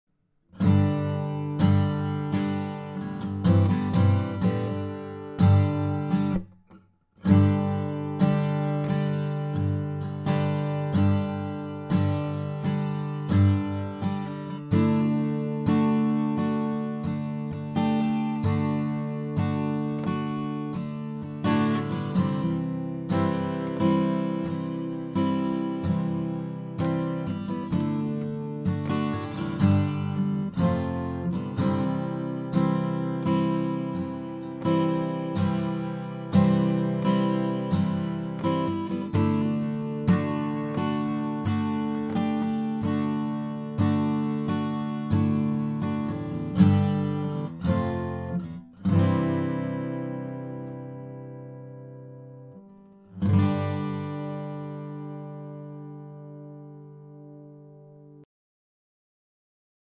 Eastman E10SS TC TFOA LTD Adirondack Natural played by Klaas Zijlstra | Demo @ TFOA
In this video our very own Klaas Zijlstra demonstrates the Eastman E10SS TC TFOA LTD Adirondack Natural. Klaas usually supports the sales-team and has proven a powerful force in keeping our large store tidy and neat. Enjoy!

2x KM184 Spaced Pair
Zoom H4NPro